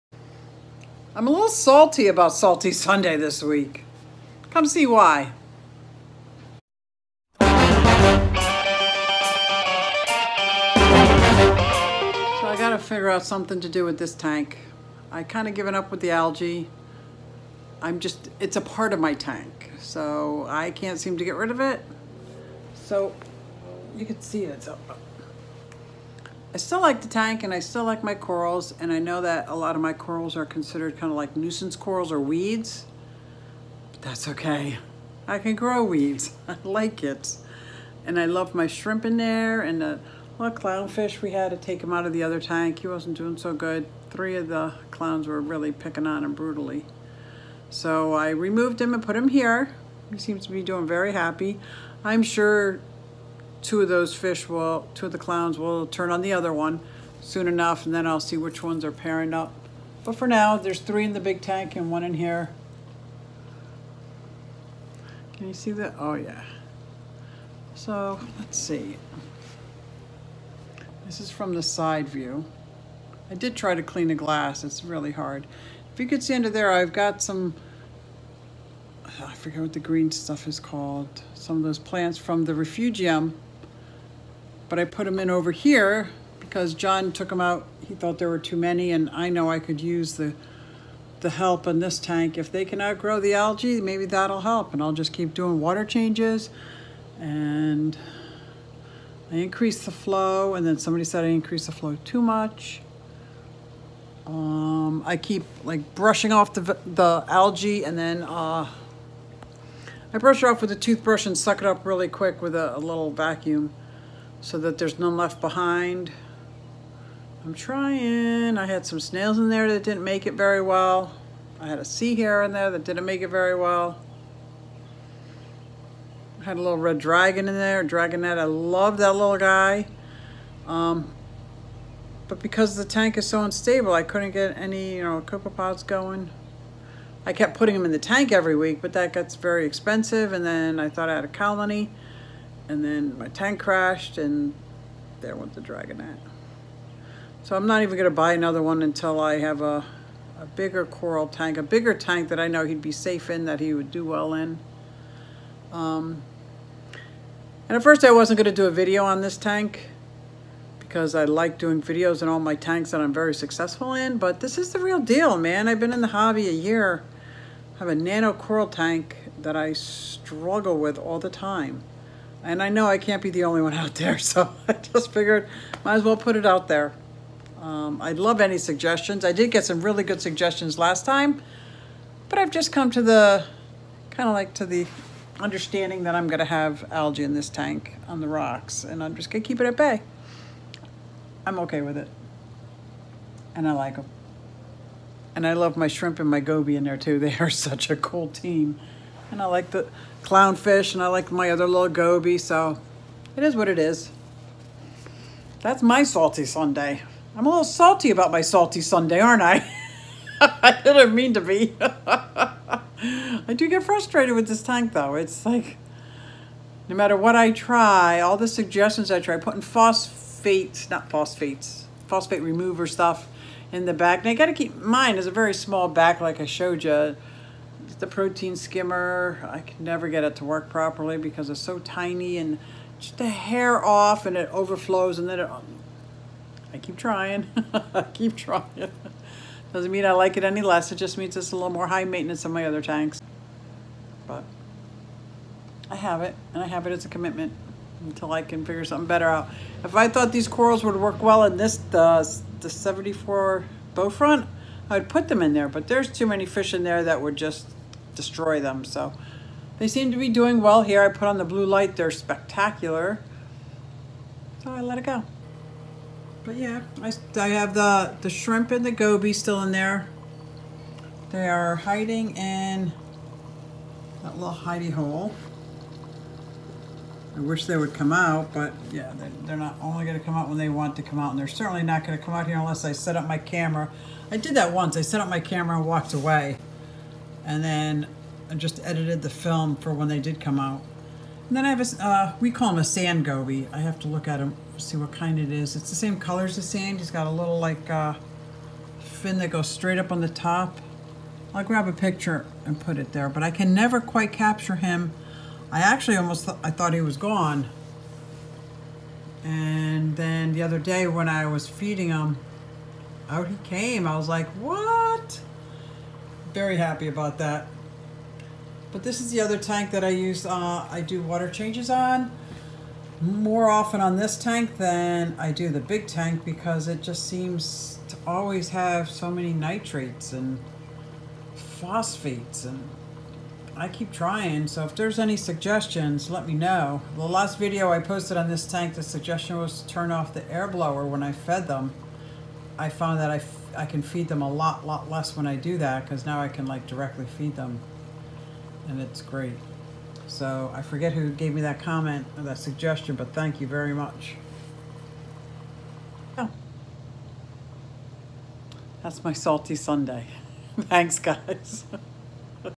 I am a little Salty about Salty Sunday Today
I have a nano saltwater reef tank that I cannot seem to get balanced. I have a bit of an algae issue all the time. I am not giving up but am feeling a bit defeated by this tank. I would love to try a large saltwater reef tank, maybe a 120 gallon? I am going to try a blackout for a bit to see if that gets it under control. I have also added a macro algae to the tank in hopes of out competing the other algae. I think it is called Caulerpa Mexicana. I will let you know if it works.